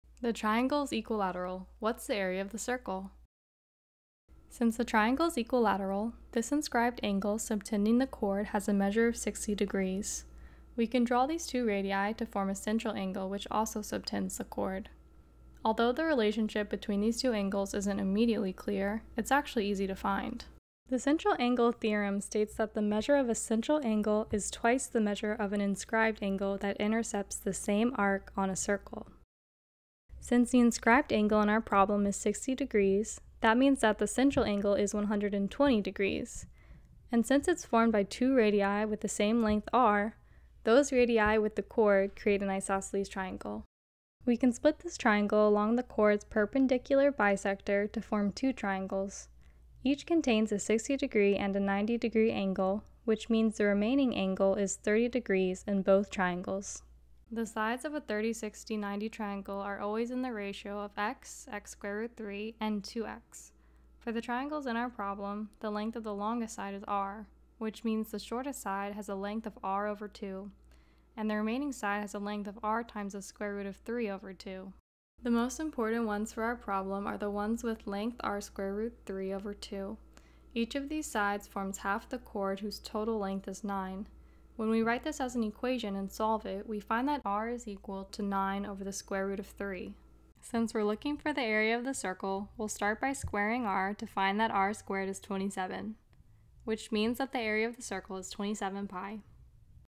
What's the area of the circle?
Connecting a circle to an inscribed triangle.